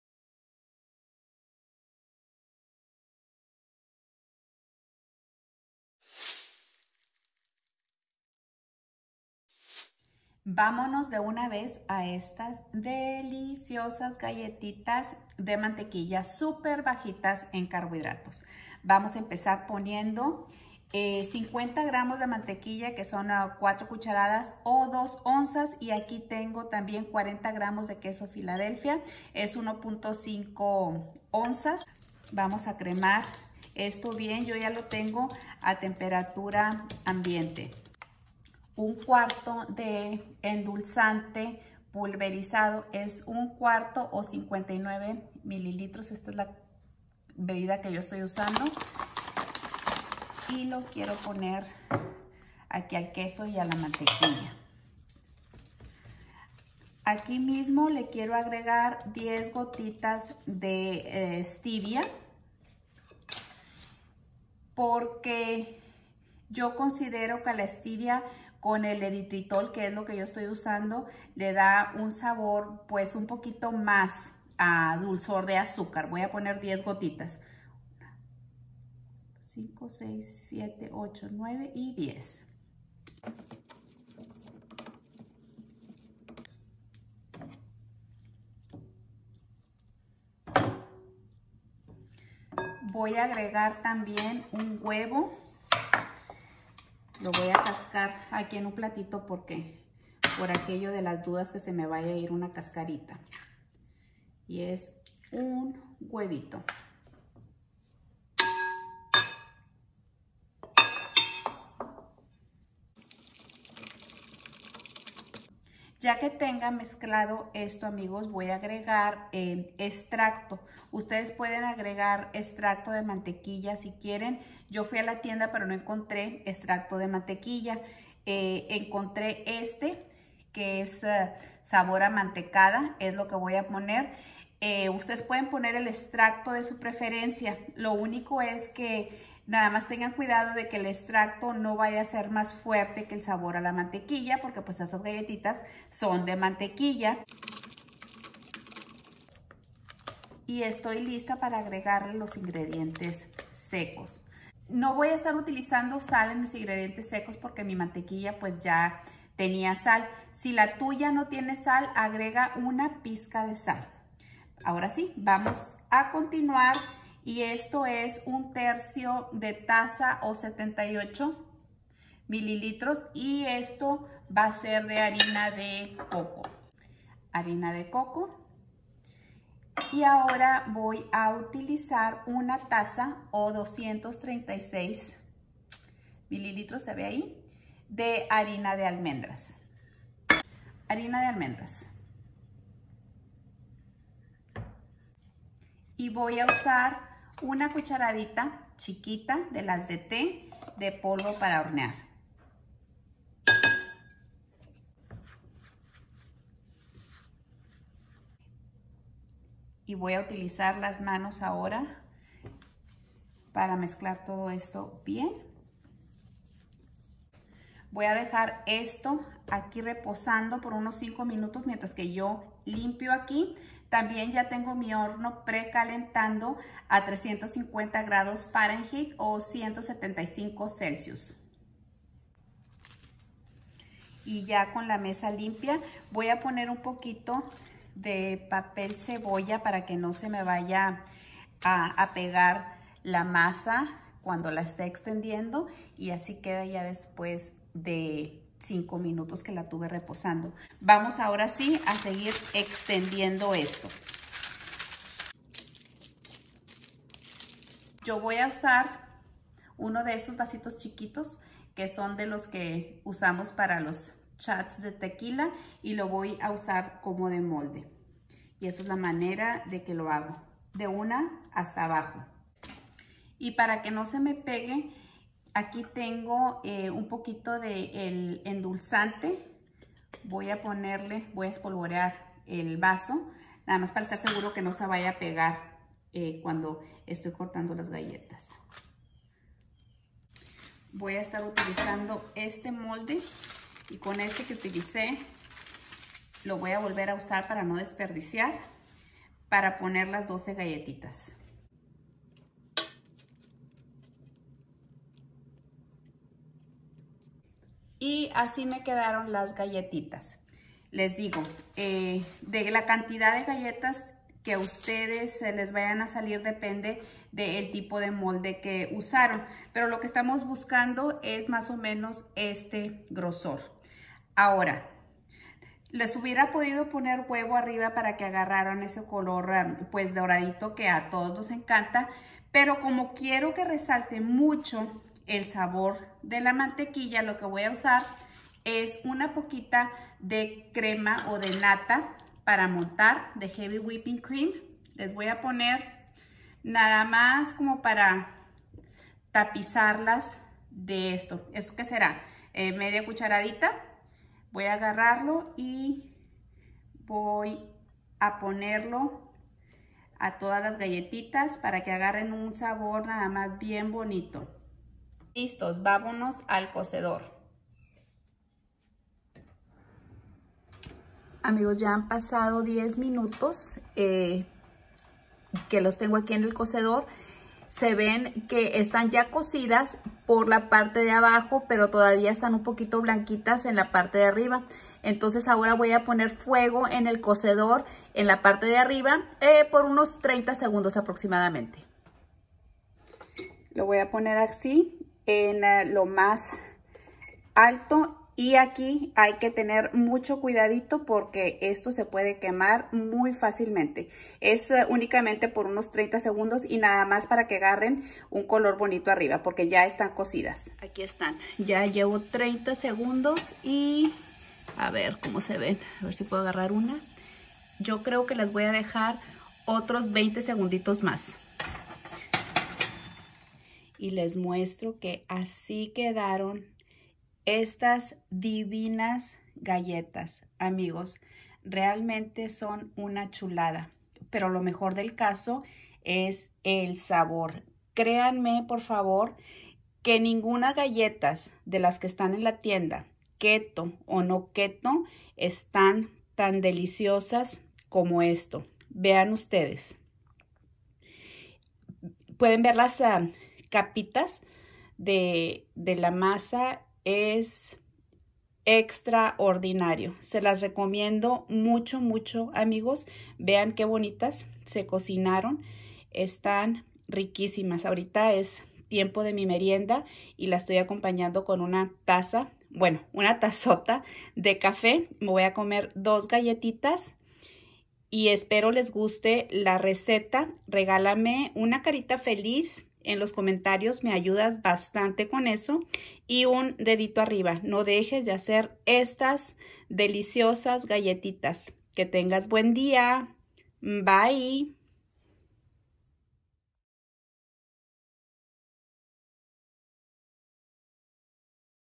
Galletas de mantequilla solo 1 carbohidrato neto
Te invito a que prepares estas deliciosas galletitas de mantequilla tienen únicamente 1 carbohidrato neto por galletita .. En mi opinión son más deliciosas que cualquier otras galletas

➡️ Precalienta tu horno a 350F/175C

👩‍🍳 Ingredientes
1 taza de harina de almendras _ 1cup of almond flour
1/3 taza de harina de coco _ 1/3 cup of coconut flour
1 cucharadita chiquita de polvo de hornear _1 teaspoon baking powder
50g de mantequilla _ 2oz butter
40g de queso crema tipo Philadelphia _ 1.5oz cream cheese
1 huevo _1 egg
1/4 edulcorante pulverizado mas 8-10 gotas de stevia _1/4cup swerve confectioners sugar substitute plus 10 drops Stevia
1 cucharada chiquita de extracto de mantequilla.(yo usé extracto de mantecada porque no tenían extracto de mantequilla en mi supermercado, puedes usar también extracto de vainilla)_1 teaspoon butter extract

➡️ Hornéalas por 10min a 350F/175C ..luego con el fuego solamente arriba por 40 segundos  para que agarren un color doradito.

🎯Macros si la divides en 16 galletas
1g carbohidrato neto
8 g grasa
2g proteina
94 calorias

y en Instagram me encuentras como Juntate con Isa